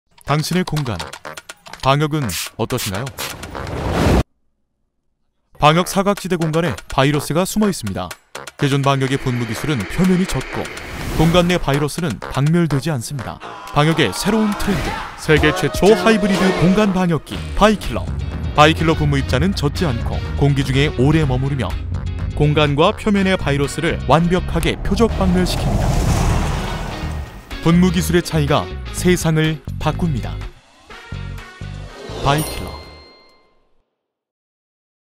[KOREA CHEM 2022] 트라코월드 / Exhibitor Introduction (TTraco World)
[ KOREA PACK & ICPI WEEK 2022 ]
KOREA CHEM 2022 참가업체 소개
참가업체 : 트라코월드

㈜트라코월드는 1986년 국내에서 불모지에 가깝던 플라스틱제 Chemical Pump류 분야의 기술지원 및 수입으로 시작하여 수년간 각종 Chemical 관련 기자재들을 원활히 공급하기 위하여 노력해 왔으며, 연구개발을 통해 축적된 기술로 PCB, LCD, 반도체, 철강, 화학, 플랜트 및 환경에 이르는 관련 기자재류의 토탈 솔루션을 수립 하였습니다.

또한 현재 Chemical Pump 및 관련 기기류, 배관기기류 등의 제조공정에 필수적인 초순수, 내식성의 특수 플라스틱제의 Pump, Valve, Pipe 및 각종 플라스틱 판재와 환봉 등 다양한 기자재류의 개발용 소재에 이르기까지 국산화 개발 및 각종 제품들을 수입, 생산 프로세스를 구축하는 필수 제품들로 높은 시장지배력과 서비스 및 기술력을 보유하고 있습니다.

2021년에는 기존에 보유중이었던 노즐 기술을 토대로 방역기 시장에 진출하여, 공간 살균 하이브리드 방역기 '바이킬러'를 출시하였습니다.

KOREA CHEM 2022 Exhibitor Introduction
Exhibitor (Company Name) : Traco World

Starting with technical support and import of plastic chemical pumps in 1986 , Traco World Co., Ltd. has been supplying various chemical-related equipment for many years. It has established total solutions for related equipment, including PCB, LCD, semiconductor, steel, chemical, plant and environment.

In addition, it has high market dominance, service and technology as essential products for localization development and production processes, including ultrapure water, corrosion-resistant special plastic pumps, valves, pipes, and other materials.

In 2021, we entered the quarantine device market based on the nozzle technology we had and launched the space sterilization hybrid quarantine device "Vi Killer".